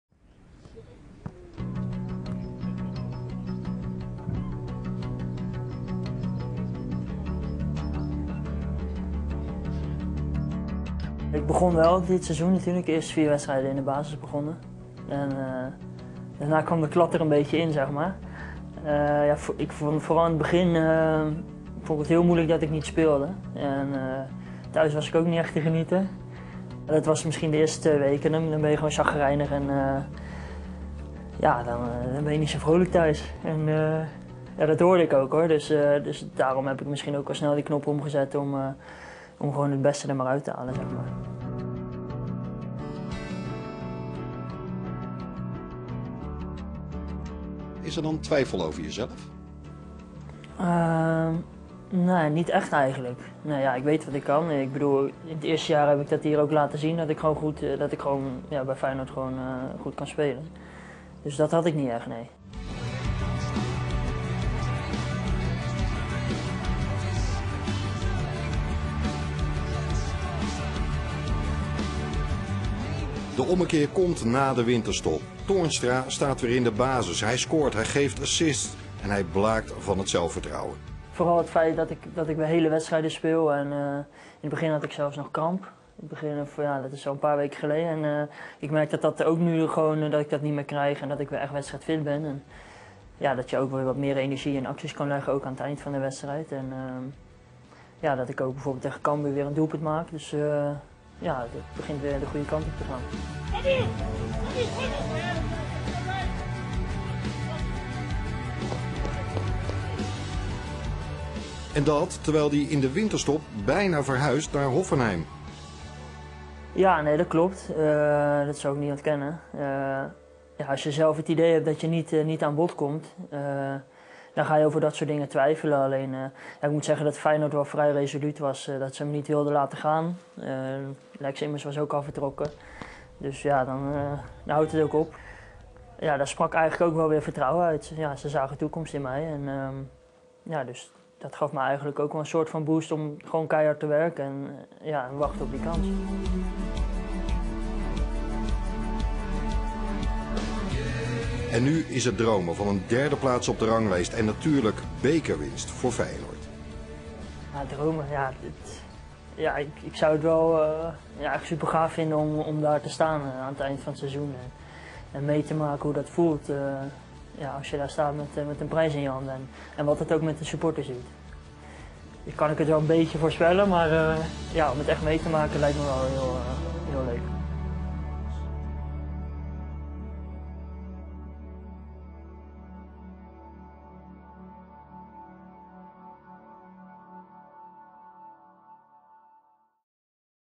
Jens Toornstra: 'Thuis was ik niet te genieten'
Voor voetballer Jens Toornstra is het een seizoen met twee gezichten. Met Feyenoord is de man uit Ter Aar op jacht naar de derde plaats, maar droomt hij vooral van de bekerwinst. En dat terwijl het seizoen voor Toornstra zo beroerd begon.